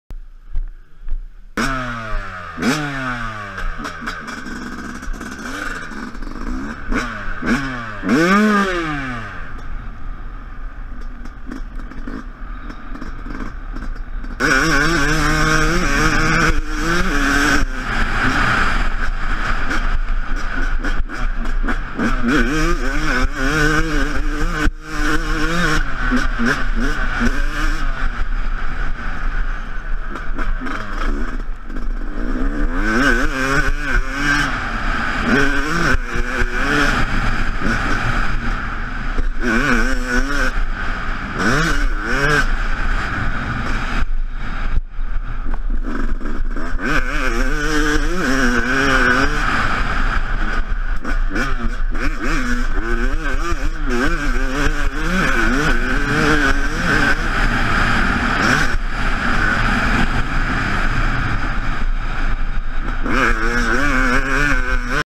20170930ヤマハ試乗会YZ250X １
２０１７年９月３０日（土）YZ250X試乗会行ってきました